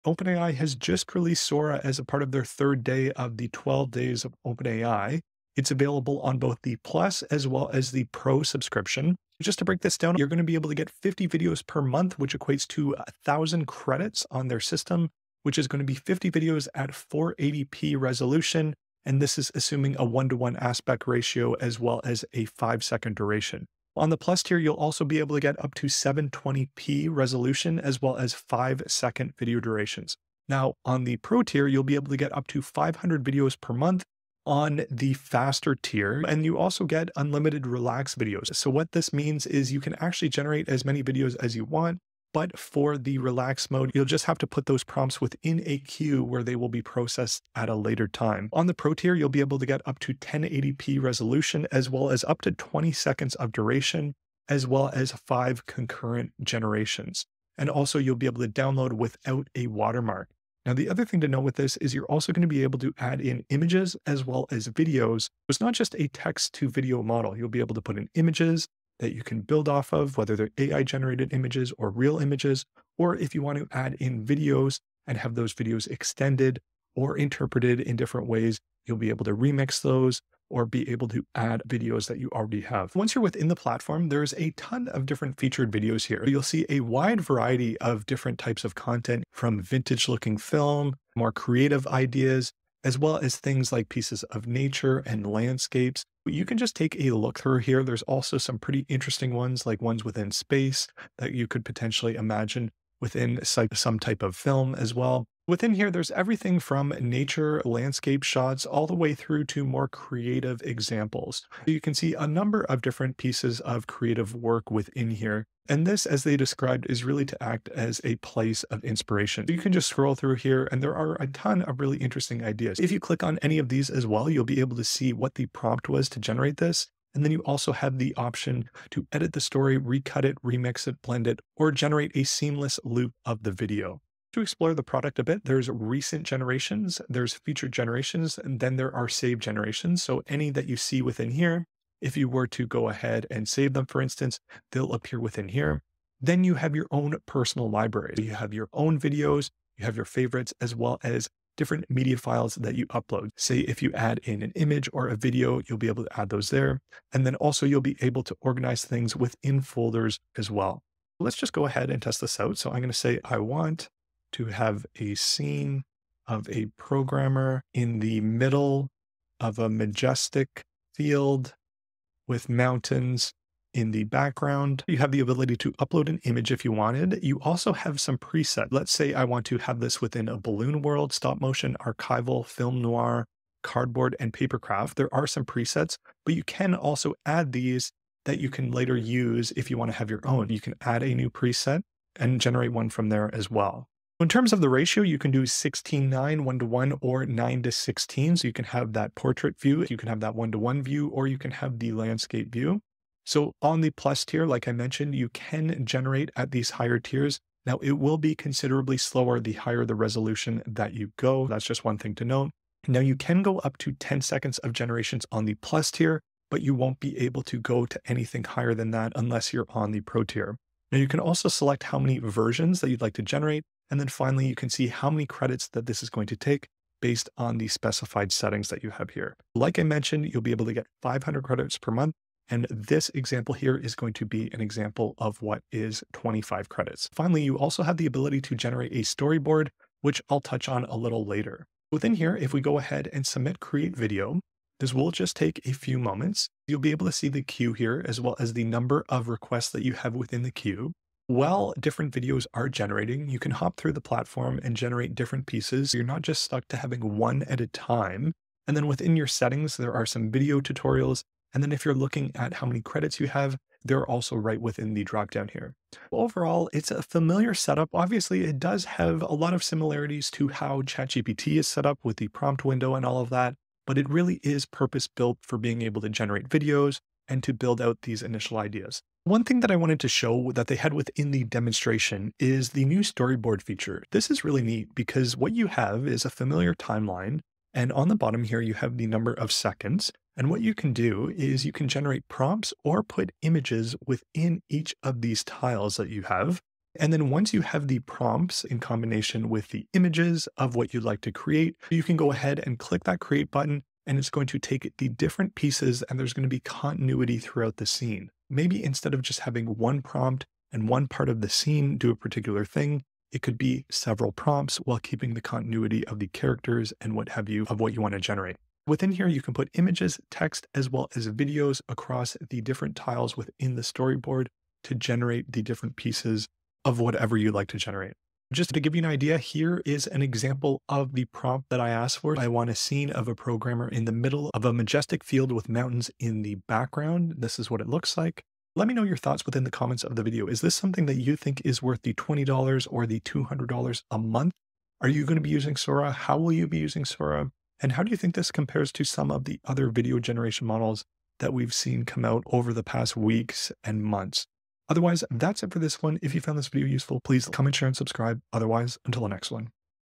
OpenAI's Sora Video Generation Model in 6 Minutes
Exploring OpenAI's New Sora Video Generator: Subscription Tiers and Features

In this video, I dive into OpenAI's newly released Sora, part of their third day of the '12 days of OpenAI'. Sora is available on both the Plus and Pro subscriptions. I explain how the system works, detailing the video generation limits, resolutions, and durations available in each tier. Plus subscribers can generate up to 50 videos per month at resolutions up to 720p, while Pro subscribers can create up to 500 videos per month at resolutions up to 1080p with unlimited relaxed mode videos. I also explore features like adding images and videos, accessing a library of featured videos, using presets and scene ratios, and the innovative storyboard feature for generating complete scenes with continuity. Watch as I provide a detailed walkthrough and even generate a sample video.

00:00 Introduction to Sora Release
00:05 Subscription Tiers and Benefits
01:00 Video Generation Features
01:22 Exploring the Platform
02:41 Creating a Custom Video
04:39 Storyboard Feature
05:38 Final Thoughts and Questions